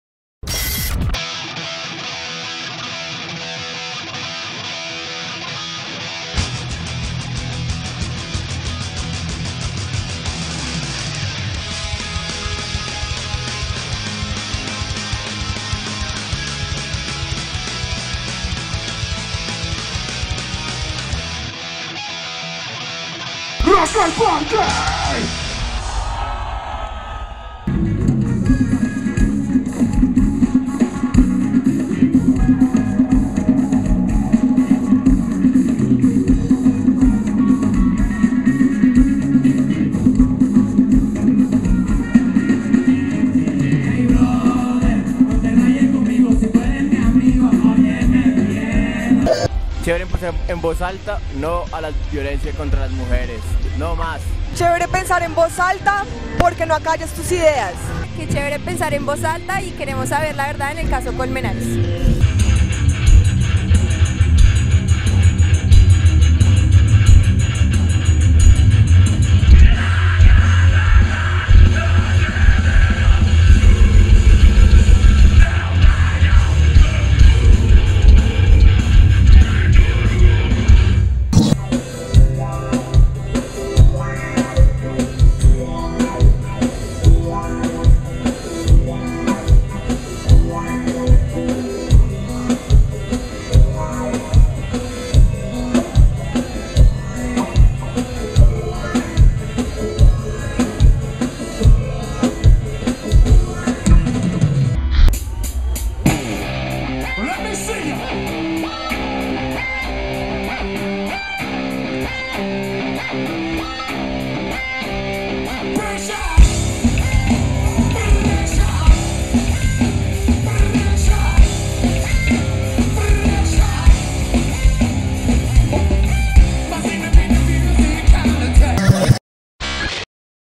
alparqueenvozalta 4
Colectro! Aire Como Plomo! Eight Bits Memory (8BM)! Skindred!
Bogotá vivió la XVIII edición del festival Rock Al Parque, realizado del 30 de junio al 2 julio de 2012 en el Parque Metropolitano Simón Bolívar.  Radiónica estuvo presente transmitiendo los tres días de festival. Capturamos estas imágenes que alimentan el recuerdo de quienes lo vivieron y, para los que por alguna razón no pudieron asistir, dibujan un panorama de lo que fue esta fiesta musical.